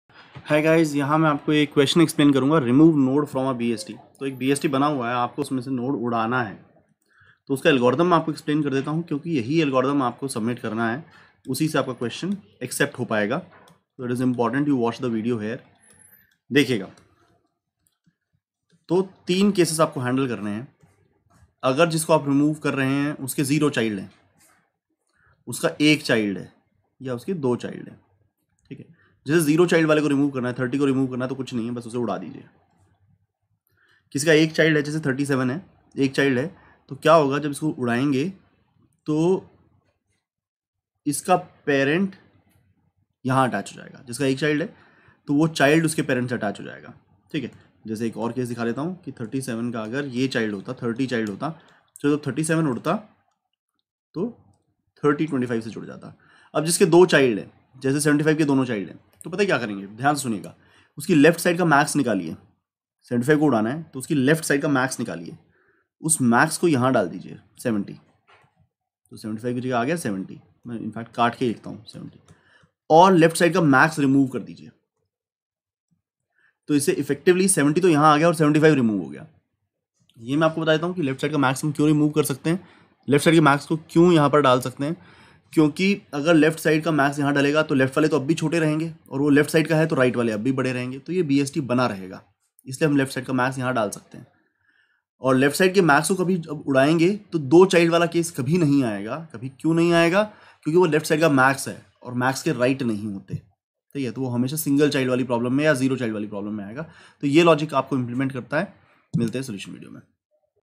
How to Remove Node in BST - Question | Binary Search Tree | Data Structure and Algorithms in JAVA
NADOS also enables doubt support, career opportunities and contests besides free of charge content for learning. In this video, we explain how to remove Node in Binary Search Tree.